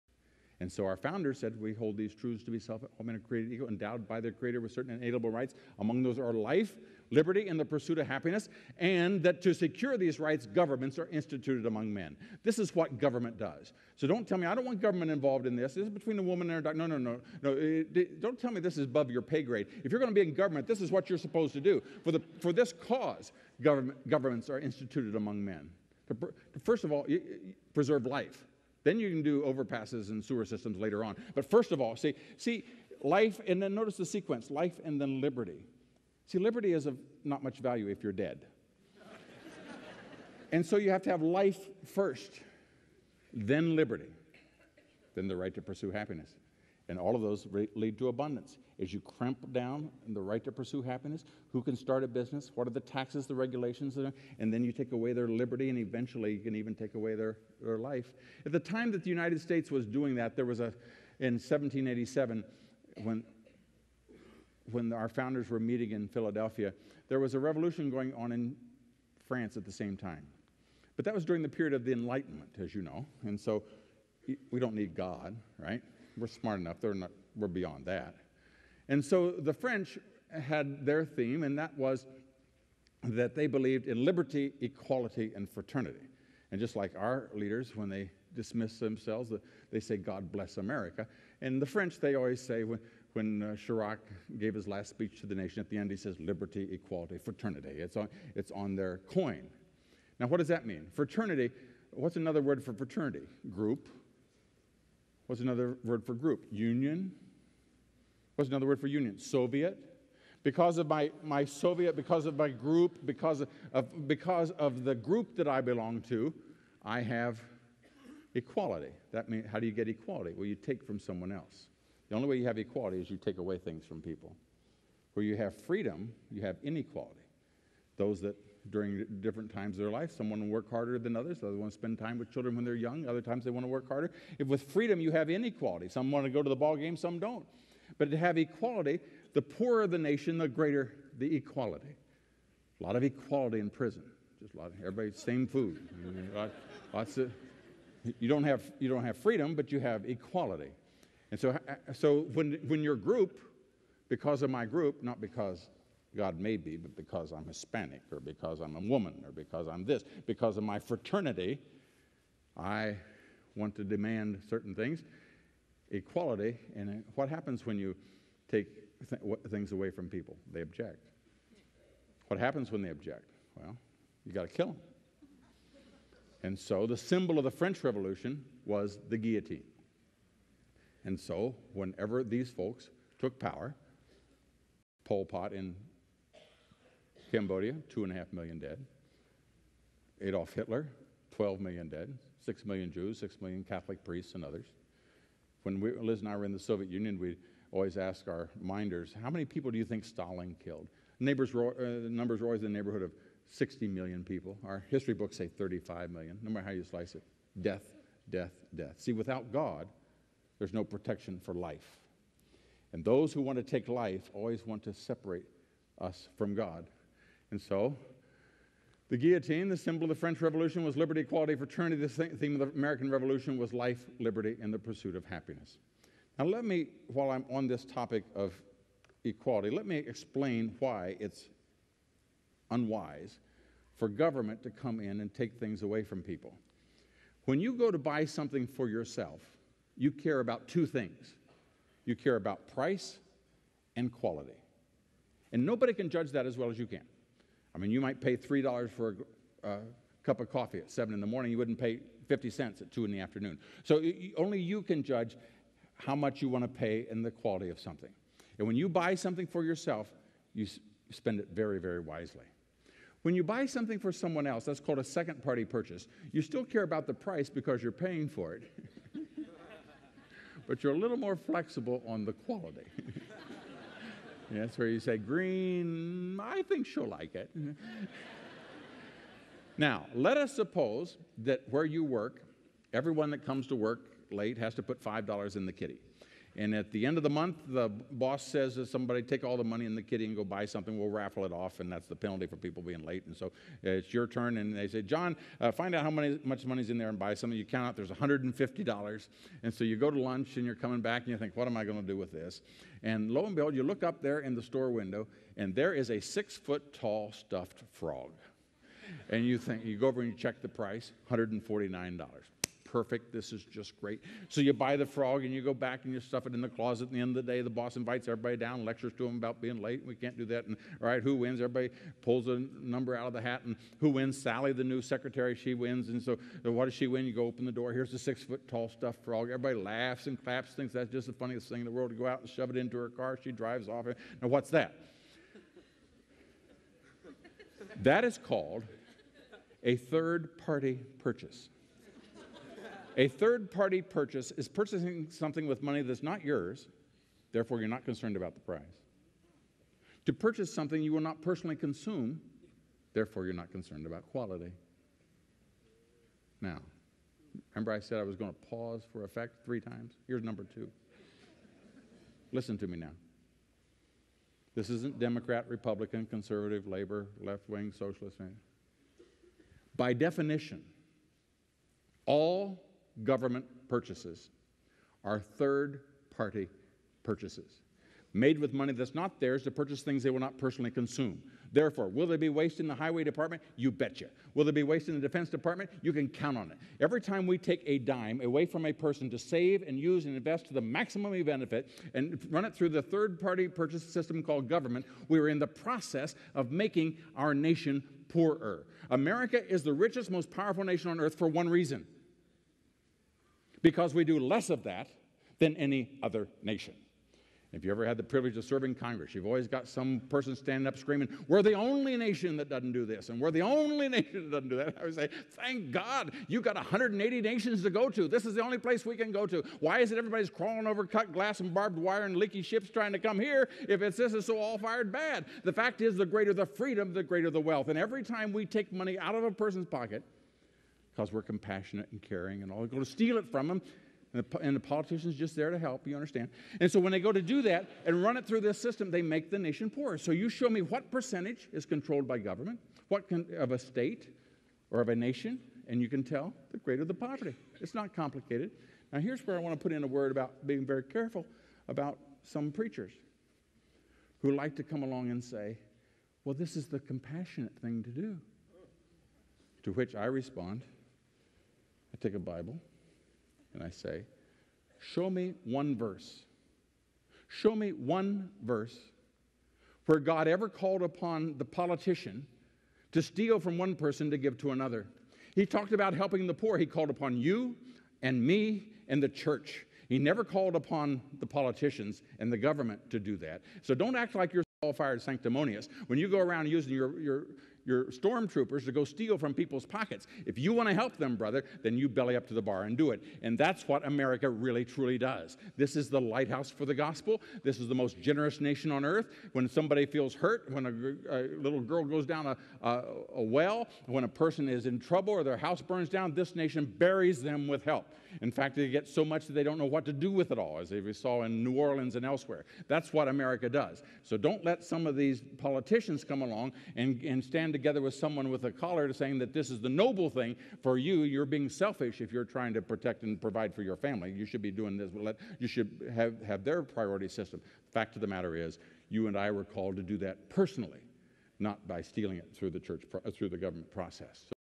Freedom In Crisis: A Worldview - part 6 of 8
Bob McEwan discusses the fact of dwindling freedoms in the face of the rising fascist police state.